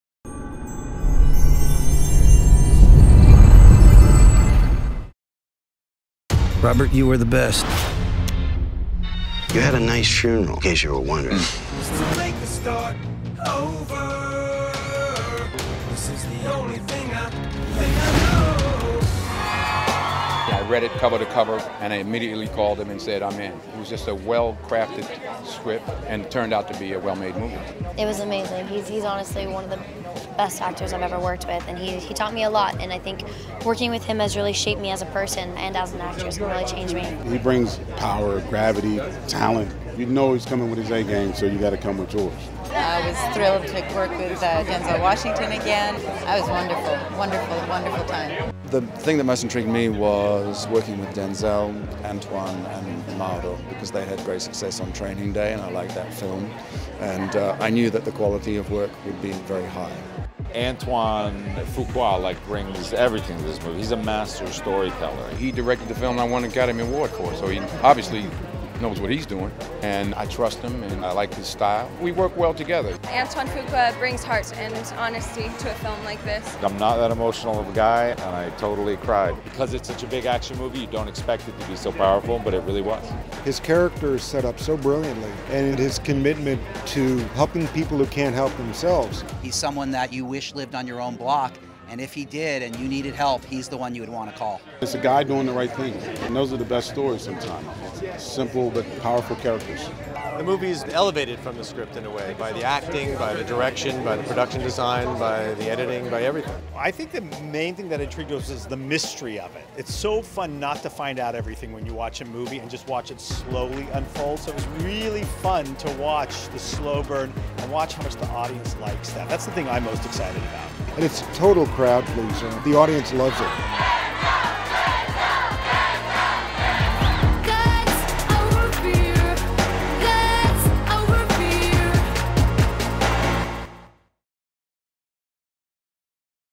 Thiện ác đối đầu (The Equalizer) - Premiere tại Toronto
Từ đạo diên của bộ phim nổi tiếng Ngày luyện tập "Training Day" và có sự tham gia của Denzel Washington. Washington vào vai Robert McCall. một quân nhân đã nghỉ hưu, McCall luôn muốn quên đi quá khứ đen tối của mình và cố hòa nhập vào cuộc sống bình thường. Tuy nhiên do phải giải cứu một cô gái (Chloë Grace Moretz thủ vai) khỏi băng cướp của Nga. McCall một lần nữa phải quay lại với quá khứ của mình